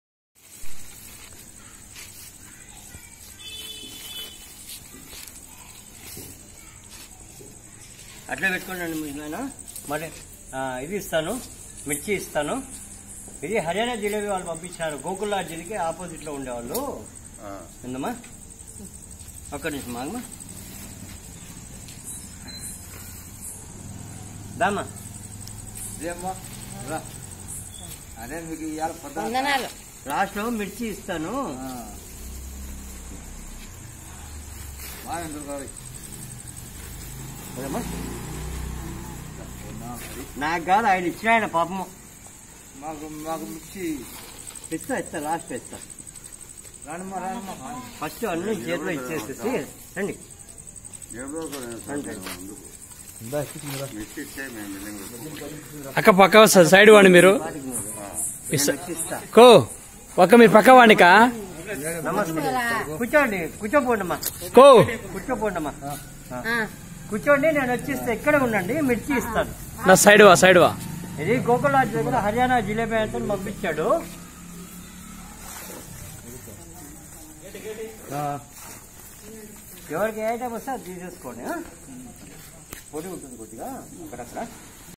రాత్రి 9 PM కు కడపలో...
**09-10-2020,రాత్రి 9 PM కు కడపలో... NAVAJEEVAN నిరాశ్రయుల ఆశ్రమంలో 40 మంది నిరాశ్రయులకు, రోడ్డుపై చిన్నపాటి పనిచేసుకునే వారికి మొత్తం 60 మందికి (150 సమోసాలు,బొండాలు,బజ్జీలు) కడప మద్రాసు రోడ్ నందు,గోకుల్ లాడ్జి దగ్గరలో ఉన్న మా కృప హర్యాణ స్పెషల్ జిలేబి & చాట్ సెంటర్ వారి సహాయంతో THUMULURU UDAYA BHASKAR అల్పాహారం ఇచ్చారు. సహాయపడుతున్న ప్రతి ఒక్కరికి సహాయం పొందుతున్న వారు తమ తరఫున కృతజ్ఞతలు తెలిపారు.**